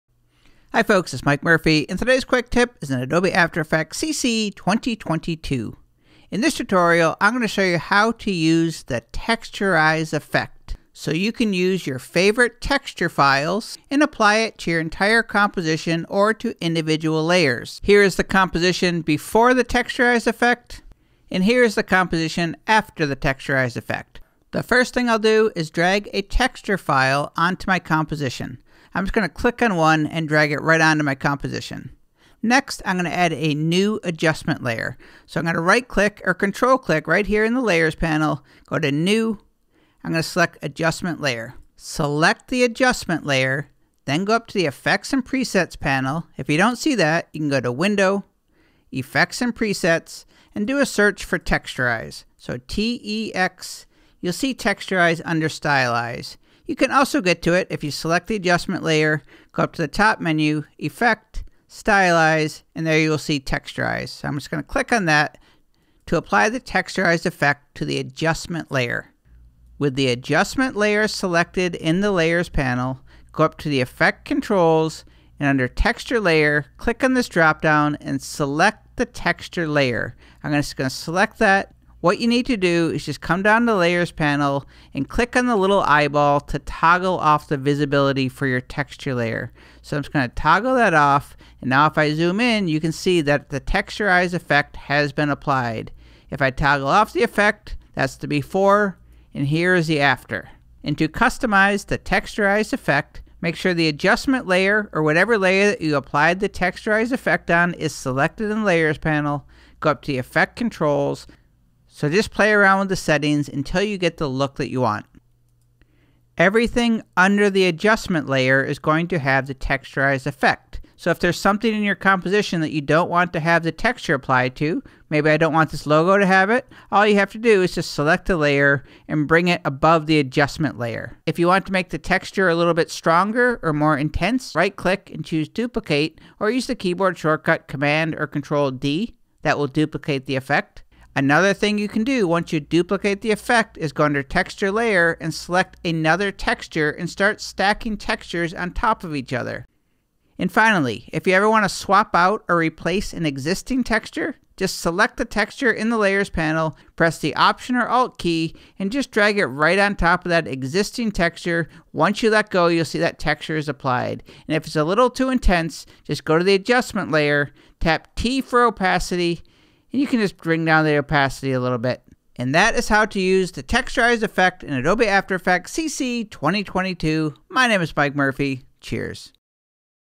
After Effects: How To Use Texturize Effect
After Effects CC 2022: How To Use Texturize Effect

This tutorial will show you how to use the Texturize Effect in Adobe After Effects CC 2022 to add textures to individual layers or to the entire Composition. The Texturize Effect is under the ‘Stylize’  Effects Category.

How To Use the Texturize Effect in Adobe After Effects CC 2022:
1. Create a New Composition (Cmd + N)
2. Drag any Texture File on the Composition Window
3. Add A New Adjustment Layer (right-click and select New…Adjustment Layer)
4. Select Adjustment Layer
5. Go to ‘Effects & Presets’ (Window…Effects & Presets)
6. Click in Search Box and type in ‘Texturize’ and it will be in the ‘Stylize’ category
7. Drag Texturize Effect on the Adjustment Layer
8. Select Adjustment Layer
9. Go to ‘Effect Controls’
10. Go to ‘Texture Layer’
11. Click the drop-down menu and select the Texture Layer to apply texture
12. Go to Layers Panel and uncheck the Eyeball 👀 to hide the texture layer
13. Adjust Settings to desired look and style you want

How To ‘Stack’ Textures or Strengthen Intensity:
1. Drag a new texture file onto the Layers Panel
2. Go to ‘Effect Controls’
3. Click on ‘Texturize’
4. Right-click and select Duplicate or press Cmd/Ctrl + D to duplicate Texturize Effect
5. The Texture should be more intense
6. Go to ‘Texture Layer’ of duplicated Effect
7. Click the drop-down menu and select the Texture Layer to apply texture
8. Go to ‘Texture Layer’
9. Click the drop-down menu and select the new Texture Layer to apply texture
10. Go to Layers Panel and uncheck the Eyeball 👀 to hide the texture layer
11. Adjust Settings to desired look and style you want

How To Replace Textures:
1. Select Texture Layer in the Layers Panel
2. Press and hold the Option or Alt Key
3. Drag a new texture file onto the existing texture